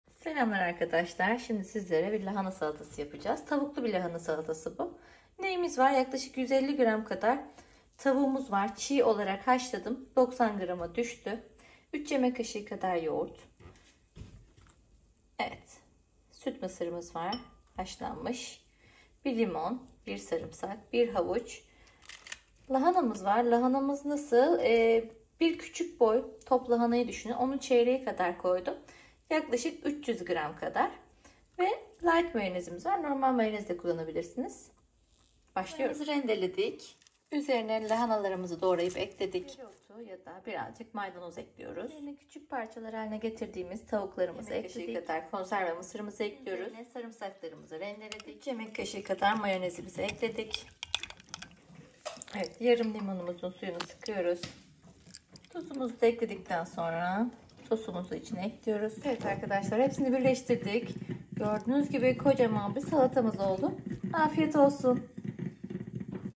Tavuklu Lahana Salatası
150 gr. Çiğ Tavuk Göğsü
1 Küçük Boy Lahananın Çeyreği(300gr.)
2 YK Haşlanmış Mısır
1 Havuç
Maydanoz
3 Dolu YK Yoğurt(Katı kısmı)
3 YK Light Mayonez
1 Sarımsak
Yarım Limon
Tuz